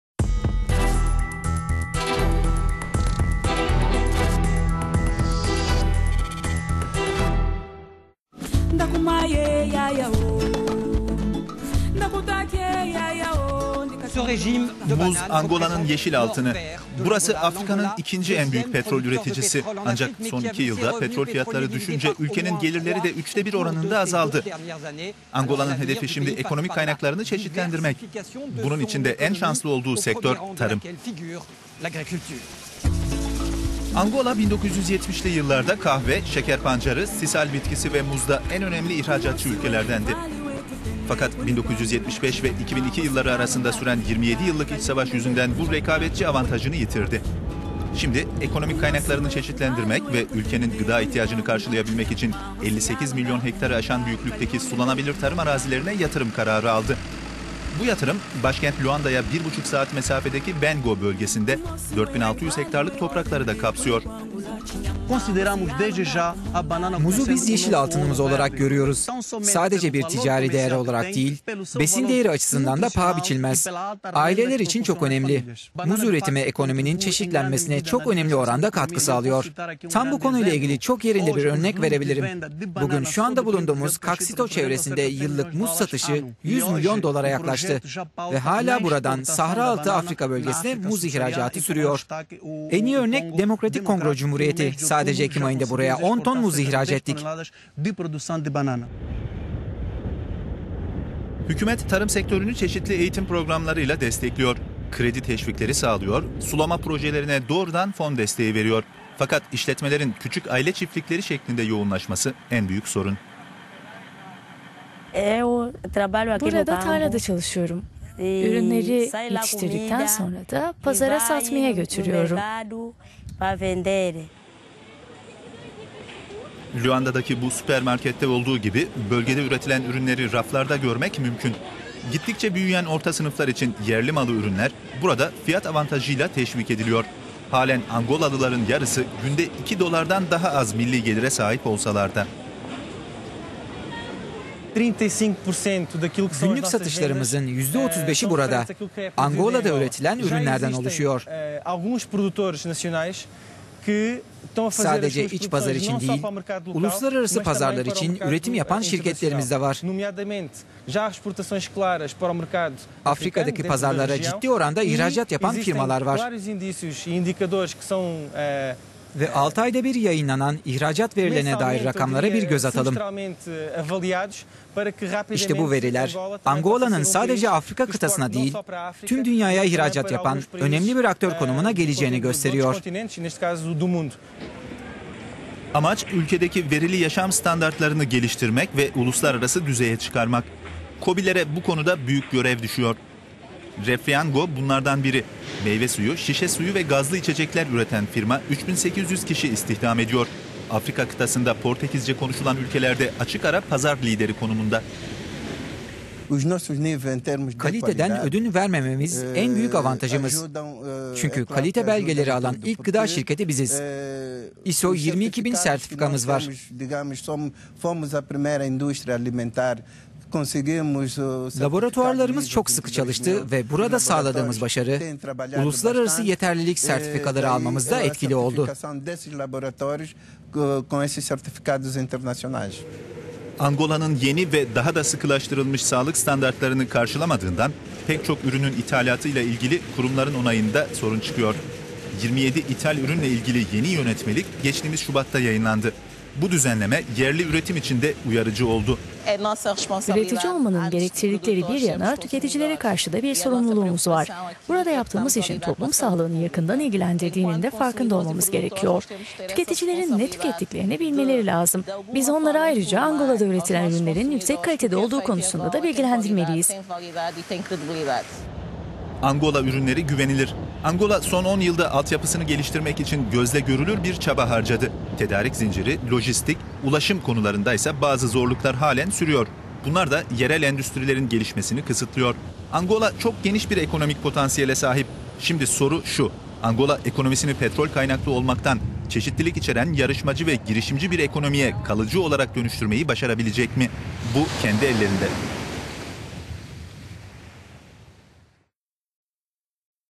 Angola tarım arazilerinde üretimi artırarak ekonomik kaynaklarını çeşitlendirmeyi deniyor - focus
Angola,  Afrika'nın ikinci en büyük petrol üreticisi. Ancak son iki yılda petrol fiyatları düşünce ülkenin gelirleri de üçte bir oranında azaldı. Angola'nın hedefi şimdi ekonomik kaynaklarını çeşitlendirmek. Bunun için en şanslı olduğu sektör ise tarım.

Angola 1970'li yılllarda kahve, şeker pancarı, sisal bitkisi, ve muzda en önemli ihracatçı ülkelerdendi. Fakat 1975 ve 2002 yılları arasında süren 27 yıllık iç savaş yüzünden bu avantajını yitirdi. Şimdi ekonomik kaynaklarını çeşitlendirmek ve ü…